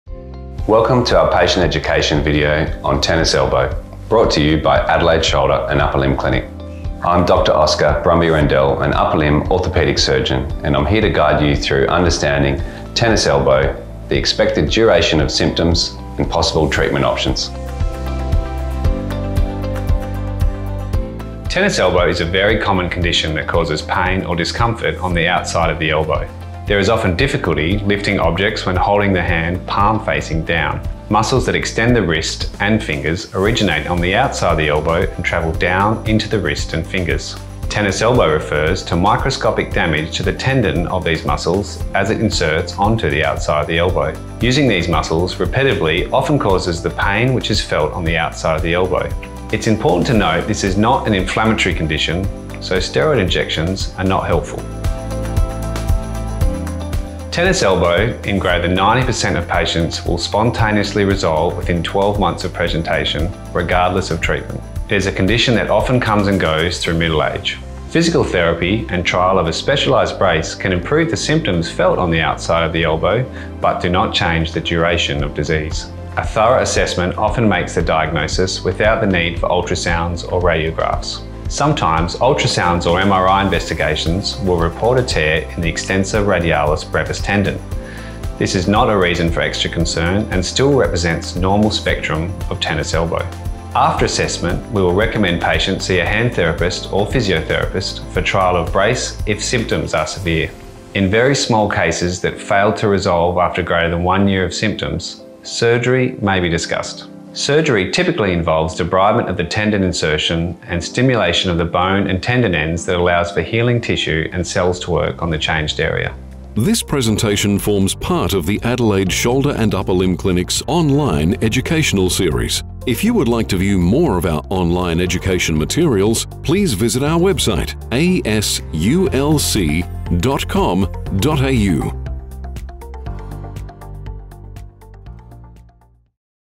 Lateral Epicondylitis or Tennis Elbow: Explained by an Orthopaedic Surgeon
In this video, Dr Oscar Brumby-Rendell takes us through the expected duration of symptoms and treatment options for lateral epicondylitis, more commonly known as tennis elbow.

Timestamps:
00:18 What is lateral epicondylitis or tennis elbow?
00:55 How is it treated?